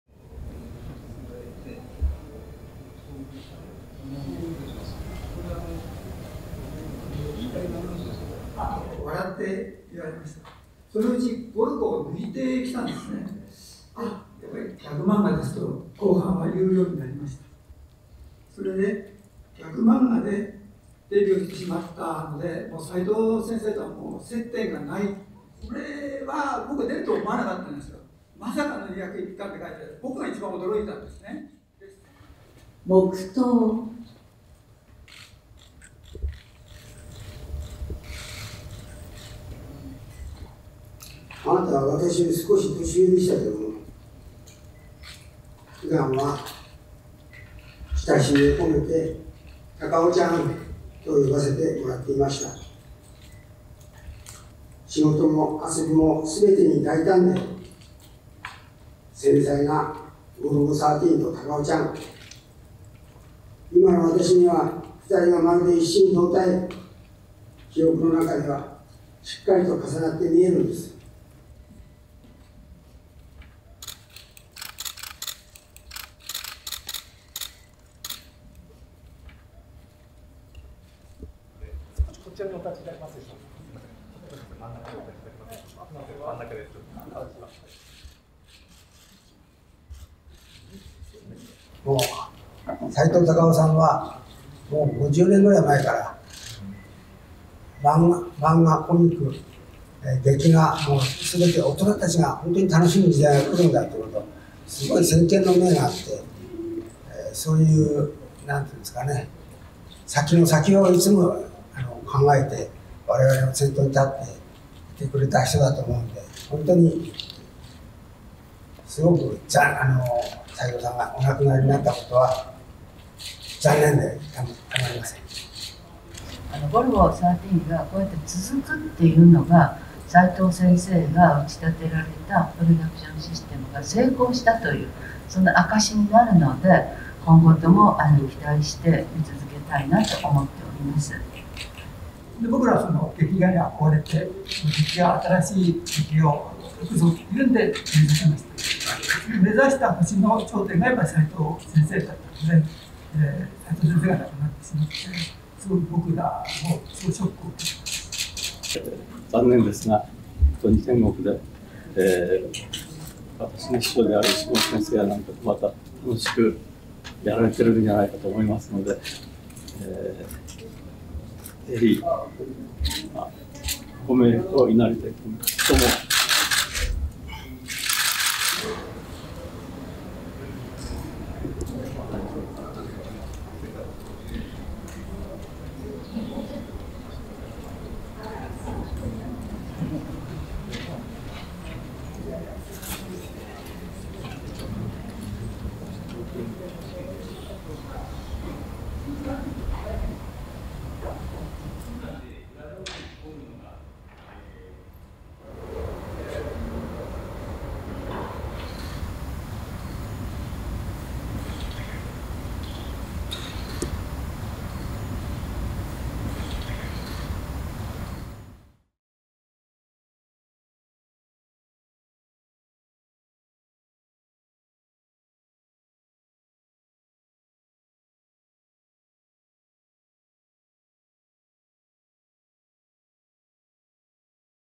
漫画家さいとう・たかをさん お別れ会
29日、東京都千代田区の帝国ホテル。「ゴルゴ13」の作者で知られ、昨年9月に死去した漫画家 さいとう・たかを さんのお別れ会があり、漫画家仲間で、「あしたのジョー」「のたり松太郎」のちばてつやさんが弔辞を読み上げた。

Golgo 13 creator Takao Saito remembered one year after death

Video footage taken on Sept. 29 in Tokyo shows a memorial ceremony for Takao Saito marking the first anniversary of the famed manga artist’s death. Mourners offered messages and testimonials before a photographic portrait of the artist surrounded by flowers and backgrounded with large images from his best-known series Golgo 13.